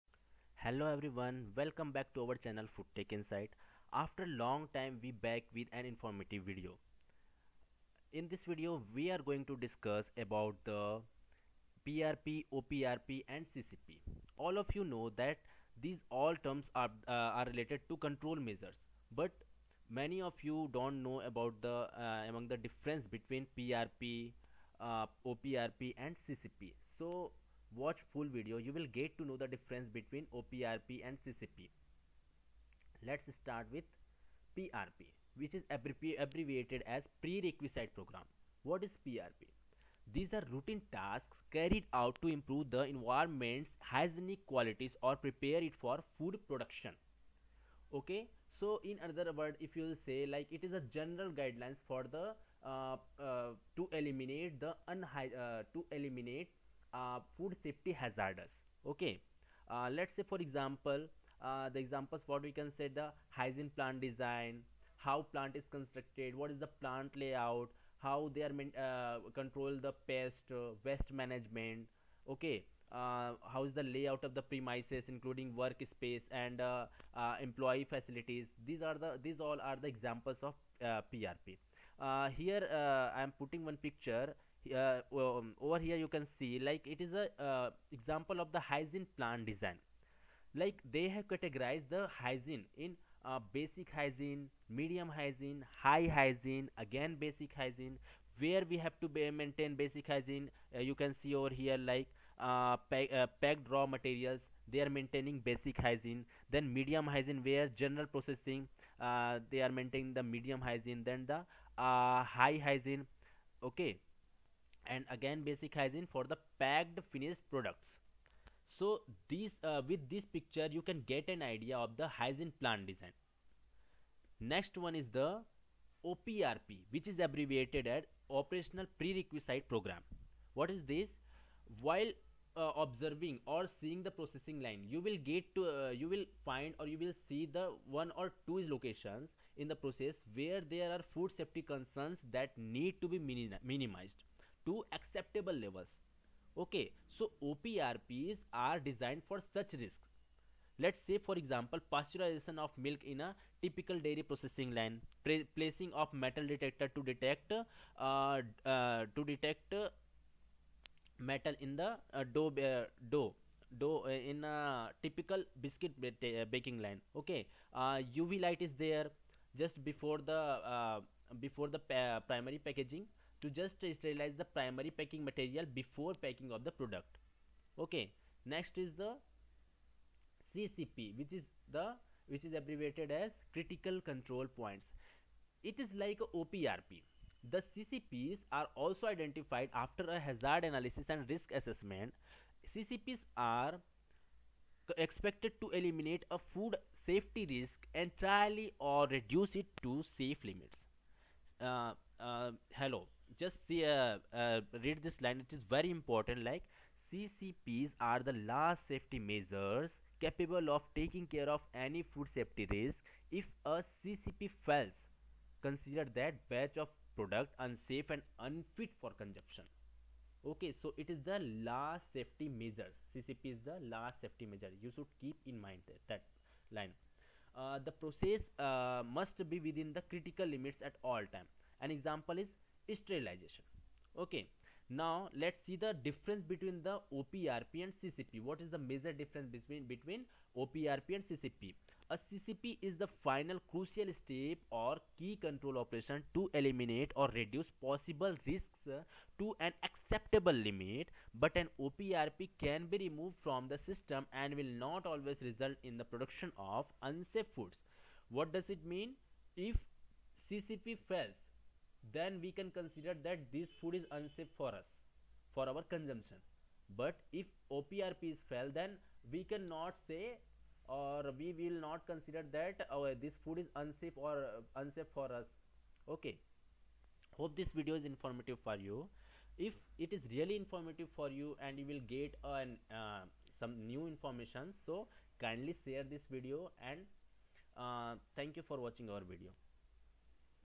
PRP, OPRP & CCP brief overview
In this video you will get the brief overview of PRP, OPRP and CCP along with practical application in industries which help you to gain practical application of these food safety related process.

For any further query and suggestion always mail-in below-mentioned id

ICAR JRF
ASRB NET
# Food process engineering